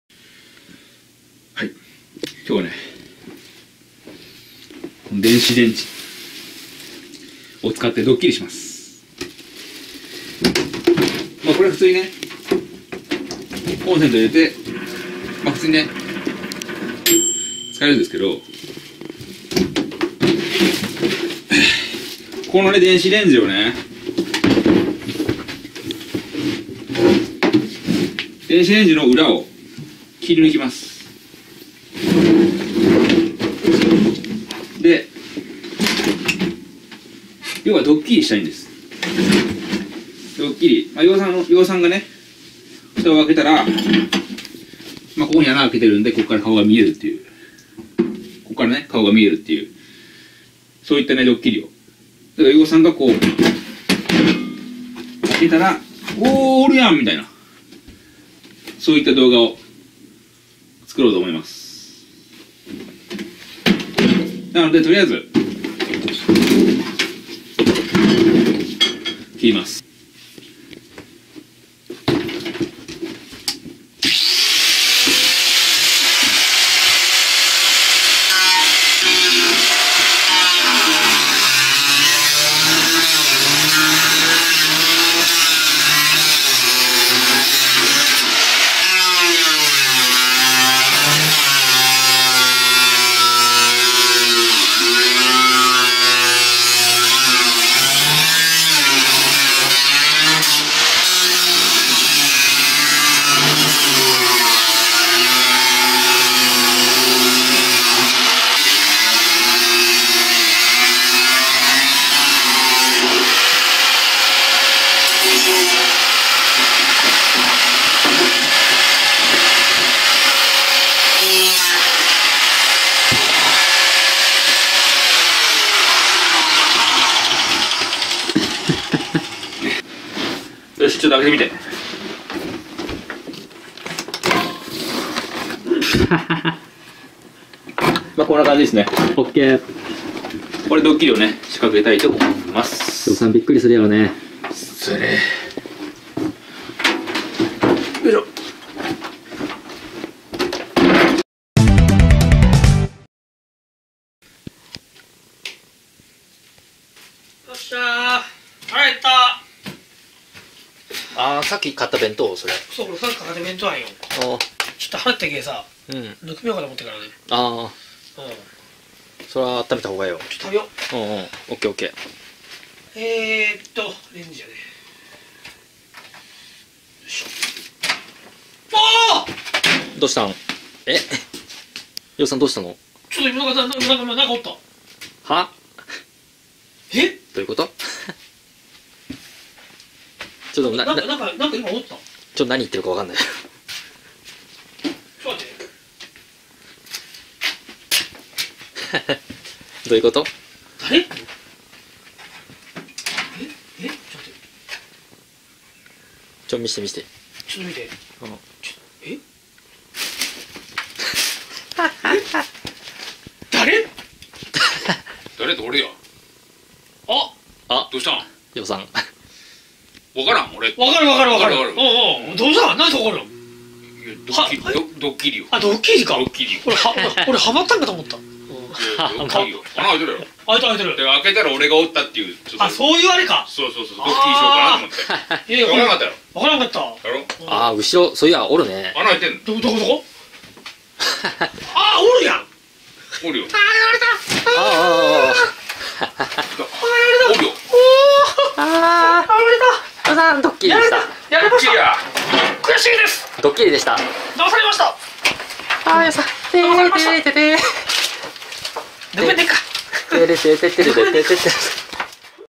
【ドッキリ】電子レンジの裏を切り取り、裏から顔を出して驚かす！
電子レンジの裏を切り取り、そこから顔を出して
友達を驚かすドッキリをしてみたいと思います。